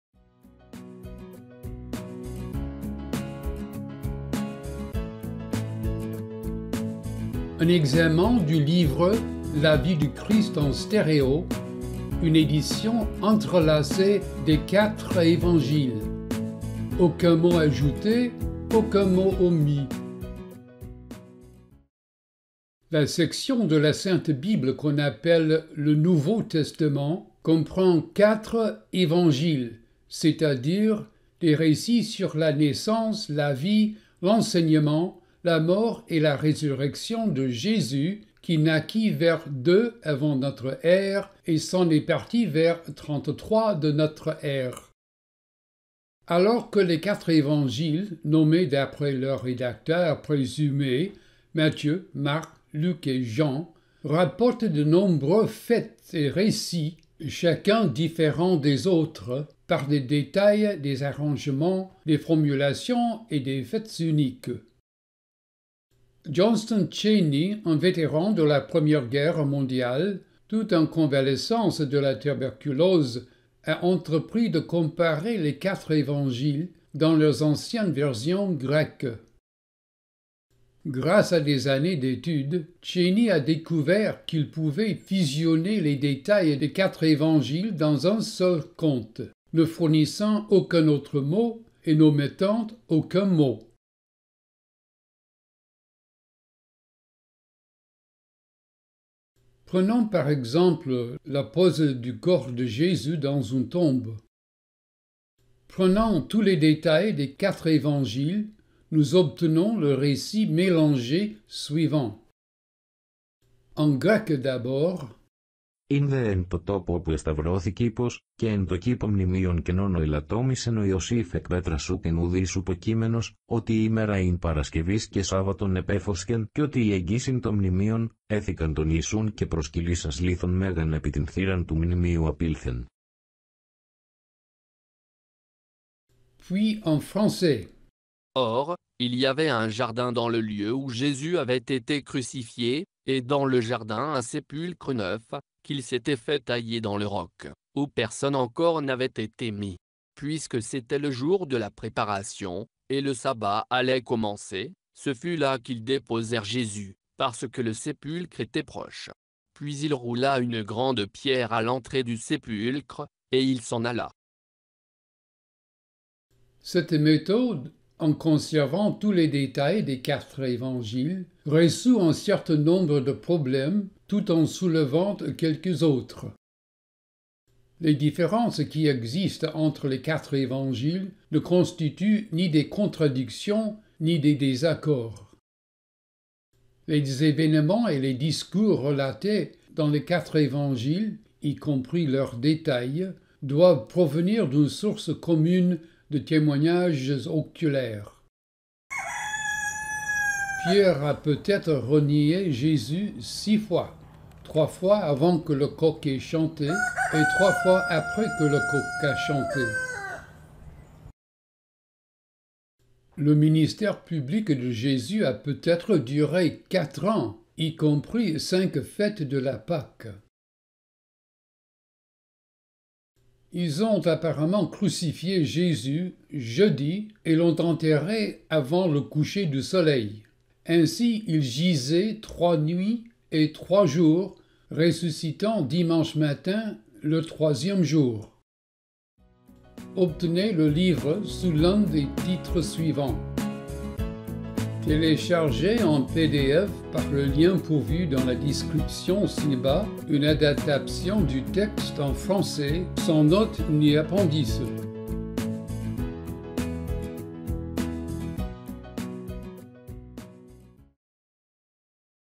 Un examen du livre La vie du Christ en stéréo
Une édition entrelacée des quatre évangiles
Aucun mot ajouté, aucun mot omis

wordwatchers.currah.us

La section de la Sainte Bible qu’on appelle le Nouveau Testament comprend quatre « évangiles », c'est-à-dire, des récits sur la naissance, la vie, l'enseignement, la mort et la résurrection de Jésus, qui naquit vers 2 avant notre ère et s’en est parti vers 33 de notre ère.

Alors que les quatre évangiles (nommés d'après leurs rédacteurs présumés, Matthieu, Marc, Luc et Jean) rapportent de nombreux faits et récits, chacun différant des autres par des détails, des arrangements, des formulations et des faits uniques.

Johnston Cheney, un vétéran de la Première Guerre mondiale, tout en convalescence de la tuberculose, a entrepris de comparer les quatre évangiles, dans leurs anciennes versions grecques.

Grâce à des années d'études, Cheney a découvert qu'il pouvait fusionner les détails des quatre évangiles dans un seul compte, ne fournissant aucun autre mot, et n’omettant aucun mot.

Cependant, lorsqu'un évangile emploie une expression précise (comme « six jours », Marc 9: 2) alors qu'un autre évangile utilise un terme général (par exemple « environ huit jours », Luc 9:26), l'expression la plus précise prévaut.

Prenons par exemple la pose du corps de Jésus dans une tombe. Prenant tous les détails des quatre évangiles, nous obtenons le récit mélangé suivant.

En grec d’abord

4 ἦν δὲ ἐν τῷ τόπῳ ὅπου ἐσταυρώθη κῆπος, καὶ ἐν τῷ κήπῳ μνημεῖον καινὸν 1 ὃ ἐλατόμησεν ὁ Ἰωσὴφ 2 ἐκ πέτρας 3 οὗ οὐκ ἦν οὐδεὶς οὔπω κείμενος 4 ὅτι 3 ἡμέρα ἦν παρασκευῆς καὶ σάββατον ἐπέφωσκεν, 3 καὶ 4 ὅτι ἐγγὺς ἦν τὸ μνημεῖον, ἔθηκαν τὸν Ἰησοῦν 1 καὶ προσκυλίσας λίθον μέγαν 2 ἐπὶ τὴν θύραν τοῦ μνημείου 1 ἀπῆλθεν.

1 Matthieu 27 :59-61. 2 Marc 15 :46-47. 3 Luc 23 :53-56. 4 Jean 19 :41-42

Puis, en français

4 Or, il y avait un jardin dans le lieu où Jésus avait été crucifié, et dans le jardin un sépulcre neuf, 1 qu’il s'était fait tailler 2 dans le roc, 3 où personne encore n'avait été mis. 4 Puisque 3 c'était le jour de la préparation, et le sabbat allait commencer, 4 ce fut là qu'ils déposèrent Jésus, parce que le sépulcre était proche. 1 Puis il roula une grande pierre 2 à l'entrée du sépulcre, 1 et il s'en alla. Section 159.

Cette méthode, en conservant tous les détails des quatre évangiles, résout un certain nombre de problèmes, tout en soulevant quelques autres.

● Les différences qui existent entre les quatre évangiles ne constituent ni des contradictions, ni des désaccords.

● Les événements et les discours relatés dans les quatre évangiles, y compris leurs détails, doivent provenir d'une source commune de témoignages oculaires.

● Pierre a peut-être renié Jésus six fois, trois fois avant que le coq ait chanté et trois fois après que le coq a chanté.

● Le ministère public de Jésus a peut-être duré quatre ans, y compris cinq fêtes de la Pâque.

● Ils ont apparemment crucifié Jésus jeudi et l'ont enterré avant le coucher du soleil. Ainsi il gisait trois nuits et trois jours, ressuscitant dimanche matin, le troisième jour.

Obtenez le livre sous l'un des titres suivants:

The Life of Christ in Stereo, Western Seminary Press, 1969.
The Greatest Story, Multnomah Press, 1994
The Greatest Life Ever Lived, Paradise Publishing, 1999

Télécharger en .pdf une adaptation du texte en français sans notes et appendices.